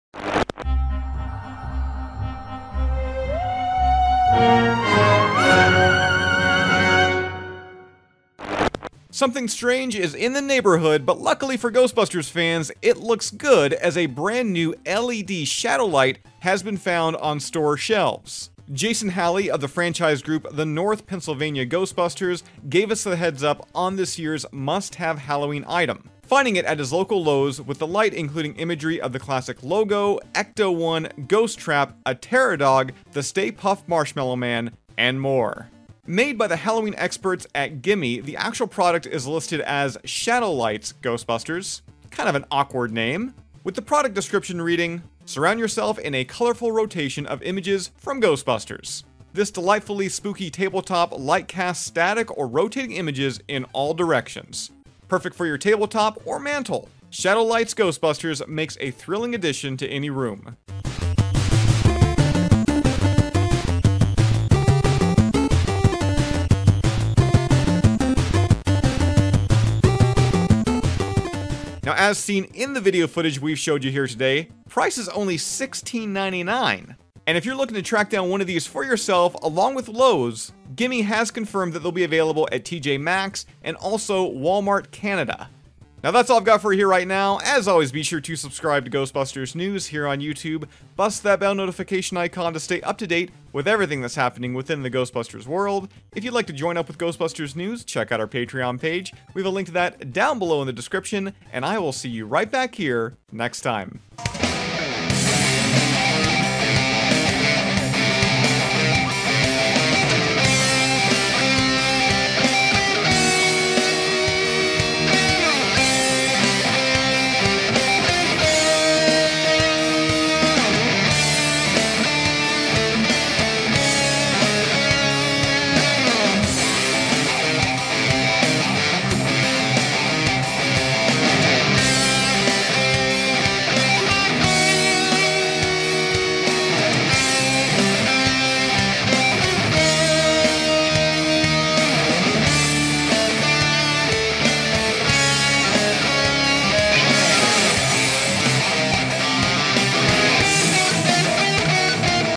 New Ghostbusters LED shadow light is this year's must-have Halloween decoration!
SEND GHOSTBUSTERS NEWS FAN MAIL:
Ghostbusters News
PMB #228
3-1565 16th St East
Owen Sound, ON N4K 5N3
Canada